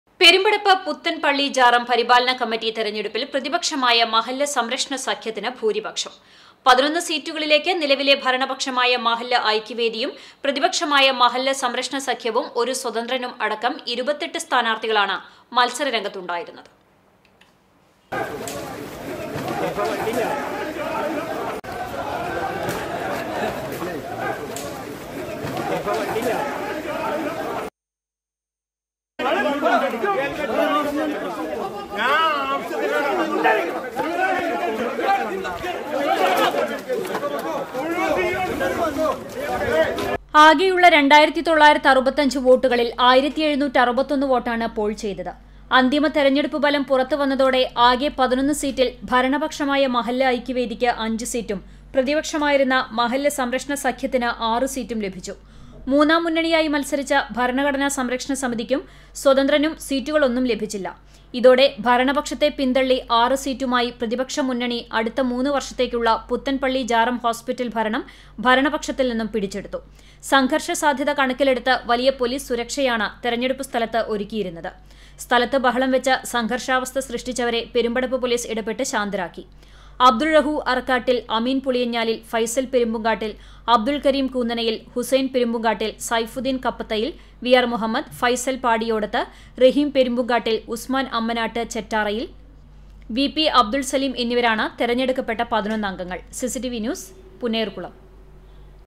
പുത്തന്‍പള്ളി ജാറം പരിപാലന കമ്മിറ്റി തെരഞ്ഞെടുപ്പ് ; മഹല്ല് സംരക്ഷണ സഖ്യത്തിന് ഭൂരിപക്ഷം
പെരുമ്പടപ്പ് പുത്തന്‍പള്ളി ജാറം പരിപാലന കമ്മിറ്റി തെരഞ്ഞെടുപ്പില്‍ പ്രതിപക്ഷമായ മഹല്ല് സംരക്ഷണ സഖ്യത്തിന് ഭൂരിപക്ഷം. 11 സീറ്റുകളിലേക്ക് നിലവിലെ ഭരണപക്ഷമായ മഹല്ല് ഐക്യവേദിയും, പ്രതിപക്ഷമായ മഹല്ല് സംരക്ഷണ സഖ്യവും ഒരു സ്വതന്ത്രനും അടക്കം 28 സ്ഥാനാര്‍ത്ഥികളാണ് മത്സരരംഗത്തുണ്ടായിരുന്നത്.